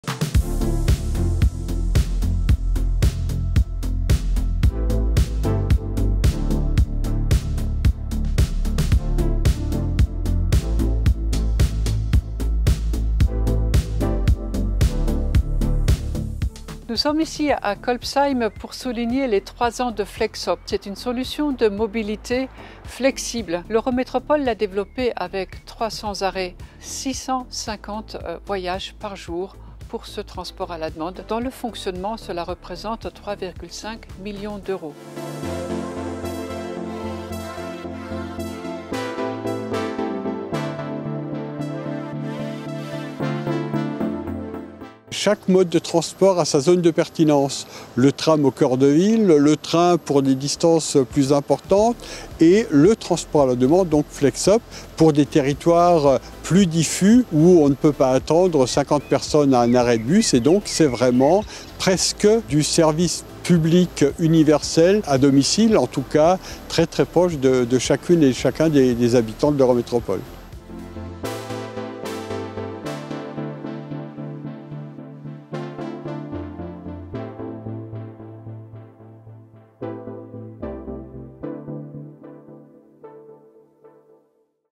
Flex'Hop fête ses 3 ans !
Expérimenté depuis 2019 et étendu à 25 communes de l'Eurométropole en 2021, le service de transport à la demande répond à une demande croissante. Avec 4500 voyages par semaine, assurés en véhicules électriques et au tarif d'un trajet en bus/tram, le dispositif élargit l'offre de mobilités alternatives à la voiture individuelle.